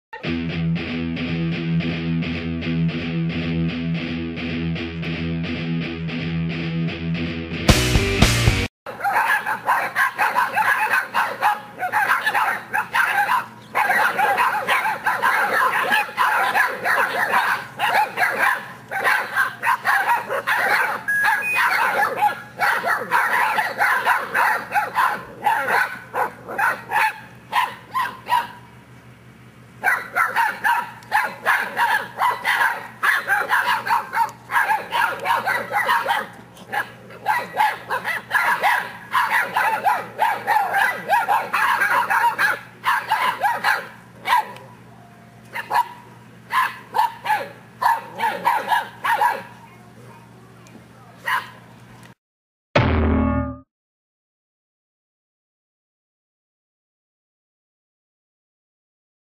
English Mastiff Puppies - "I bark, therefore I am"
English Mastiff Puppies - Geronimo x Edie puppies barking at 7 weeks old.